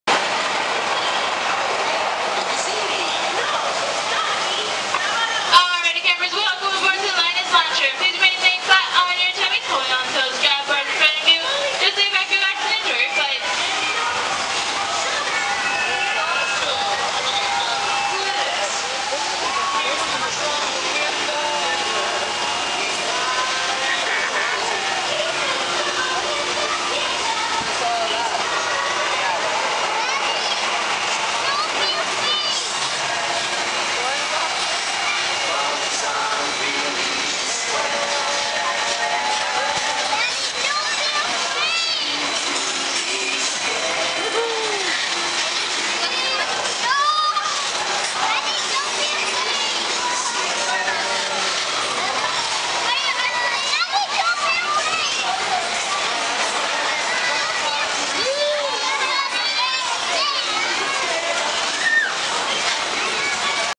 Linus Launcher
New ride in Camp Snoopy at Knott's Berry Farm